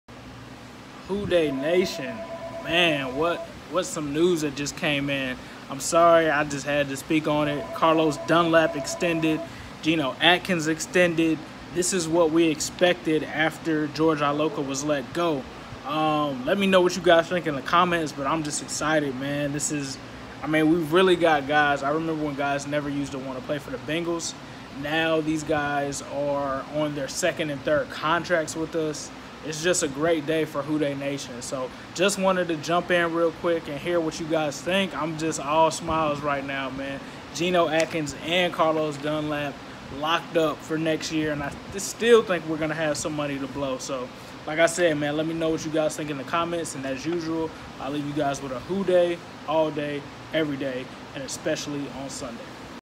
Carlos Dunlap & Geno Atkins Extended Reaction
Bengals extend both Geno Atkins and Carlos Dunlap! What a time to be alive!